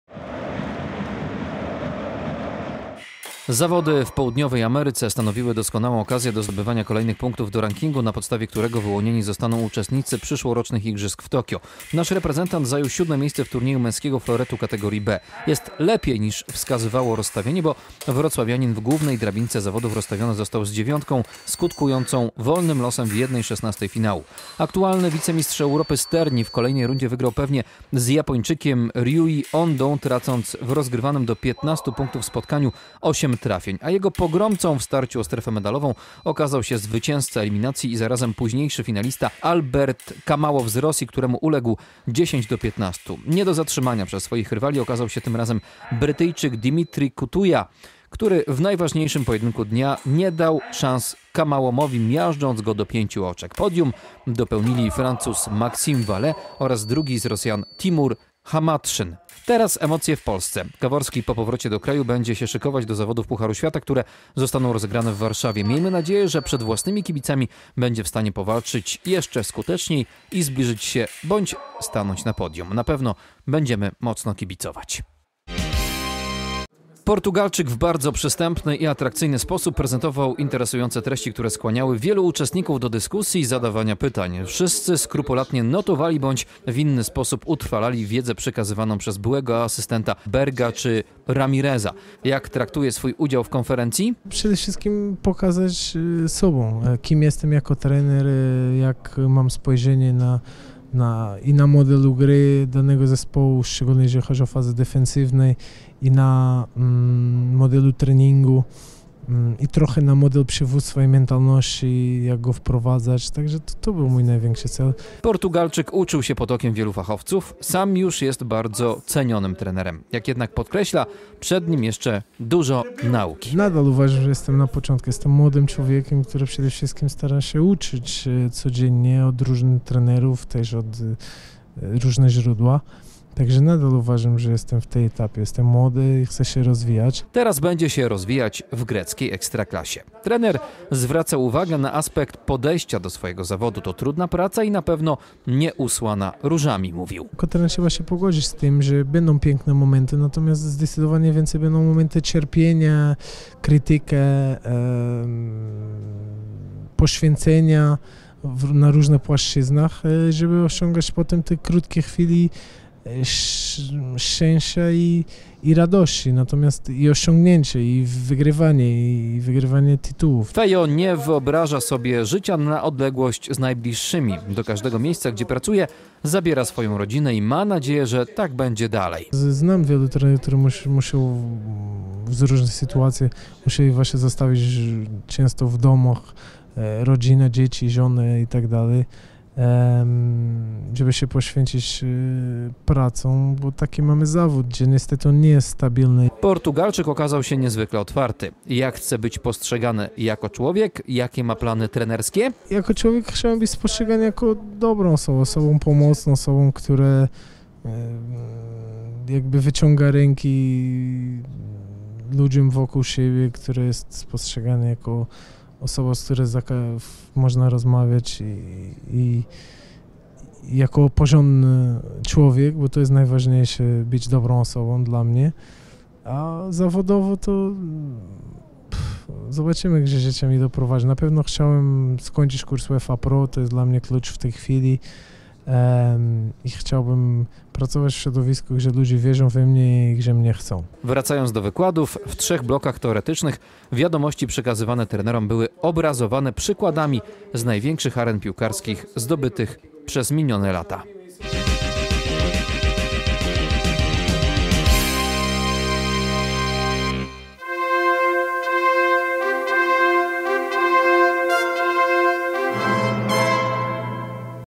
Sportgame 28.05.2019
Jacek Gaworski potwierdził przynależność do światowej czołówki. Wrocławianin zajął siódme miejsce w zawodach Pucharu Świata w Brazylii. Teraz szermierze na wózkach zjadą do Warszawy. W ostatnią niedzielę maja w sali konferencyjnej wrocławskiego MCS-u oraz na terenie Stadionu Olimpijskiego odbyła się druga edycja Ślęza Conference. Uczestniczył w niej m.in. portugalski trener Goncalo Feio.